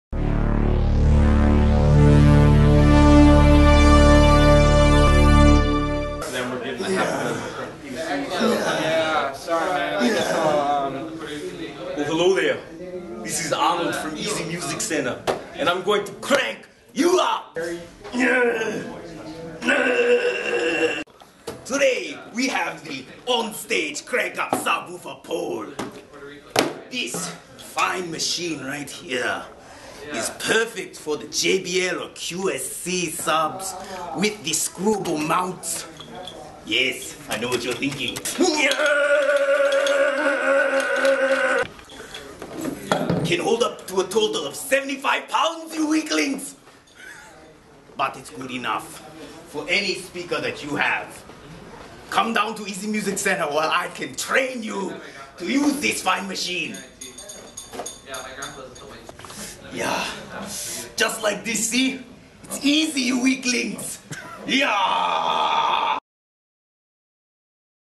On-stage SS7747 Crank-Up Subwoofer Pole
Arnold shows us the On-stage SS7747 Crank-Up Subwoofer Pole.

Features:

- Easy to use hand crank makes height adjustments effortless
- Adjusts height of pole from 35 to 45 inches
- Shaft made from heavy-duty aluminum
- Can support speakers up to 75 pounds
- Eliminates lean and adds stability to subwoofer satellite speaker systems